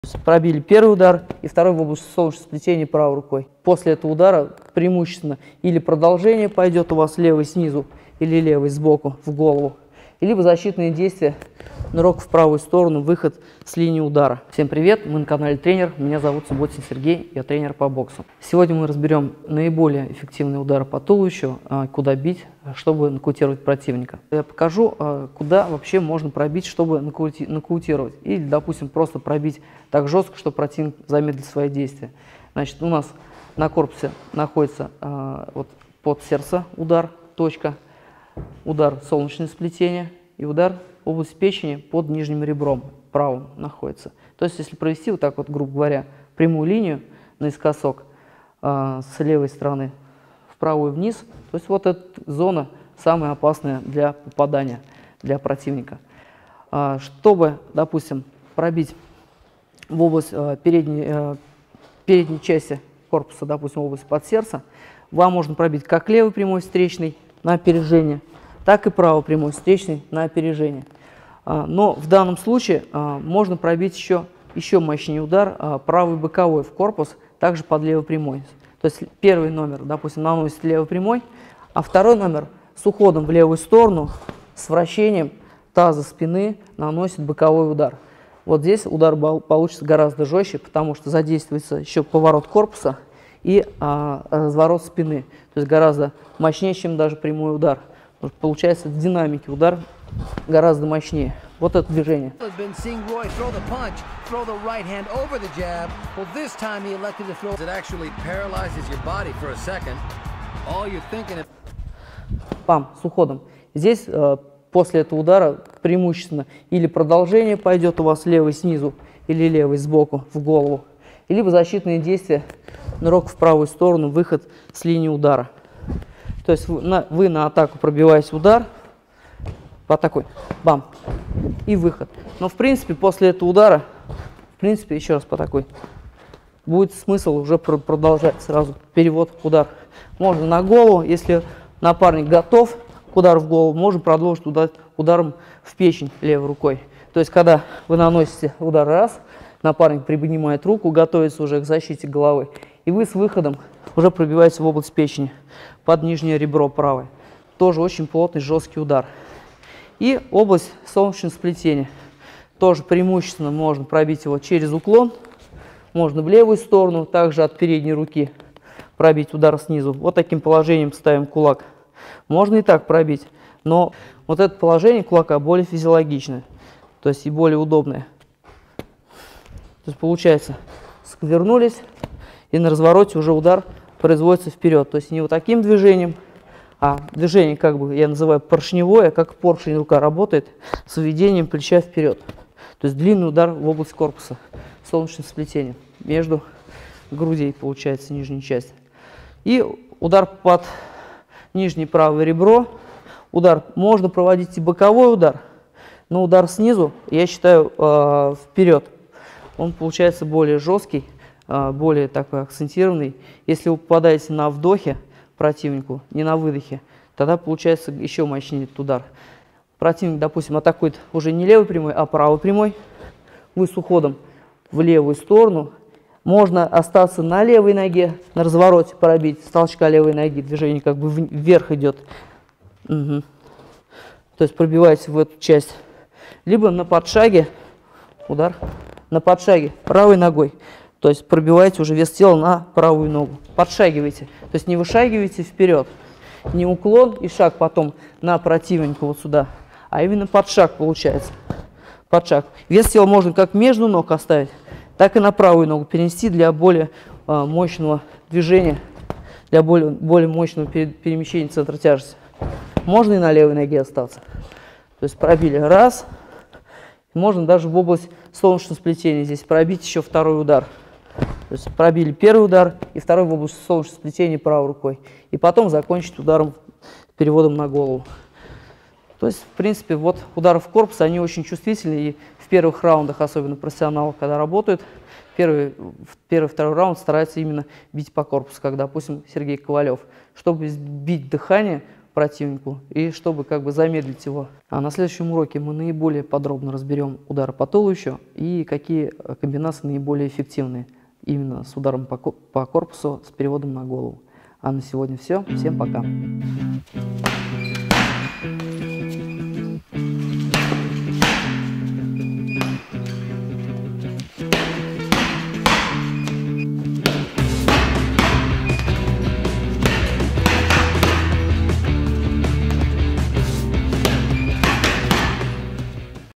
Куда бить для нокаута по туловищу
В этом уроке мы покажем куда бить для нокаута по туловищу.
Удары по туловищу очень болезненные и если не нокаутируют соперника, то причинят много боли и ваш противник потеряет много сил. В туловище нужно бить в начале боя чтобы противник быстрее устал и не смог бить и защищаться. Если вы научитесь правильно наносить эти удары, то ваша техника станет более разнообразной. Ударом по туловищу можно подготовить нокаутирующий удар в голову, а также ударом по печени или под сердце можно послать в нокаут соперника. Многие бойцы могут выдержать удар по голове, но удар по печени гораздо болезненнее.